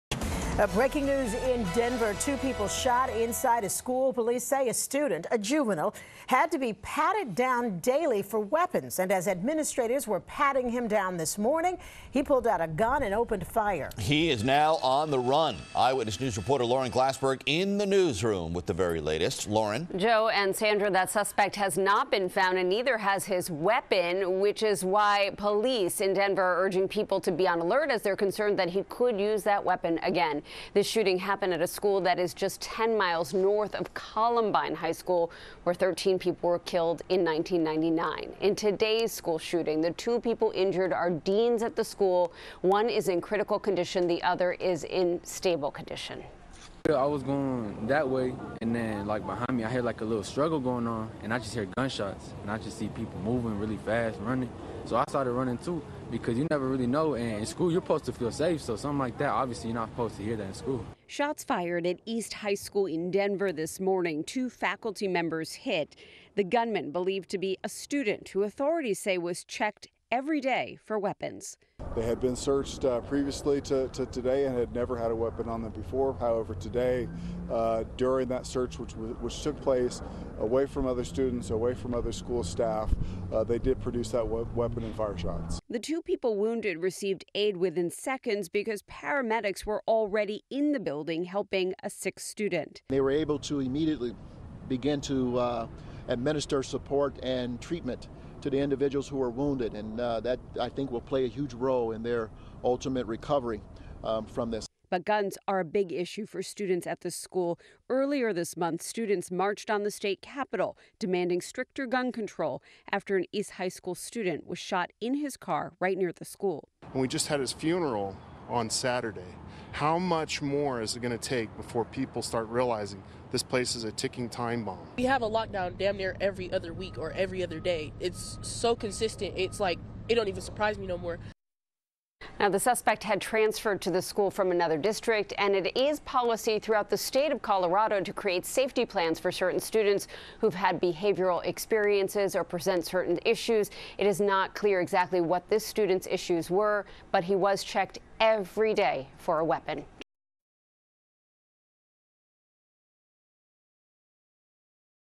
Student allegedly shoots, wounds 2 staffers at Denver high school
A student who was under a "certain agreement to be patted down each day" at school allegedly shot and wounded two school administrators at East High School in Denver, authorities said.

The suspect, 17-year-old Austin Lyle, fled the school after the Wednesday morning shooting and a search for him is ongoing, Denver police said.

The handgun used in the shooting has not been recovered, and police warned the public to not approach Lyle, calling him armed and dangerous. Lyle, who is wanted for attempted homicide, may be driving a 2005 red Volvo XC90 with Colorado license plate BSCW10, police said.

Both faculty members are in stable condition, according to the hospital. One underwent surgery and the second was able to speak to authorities, officials said.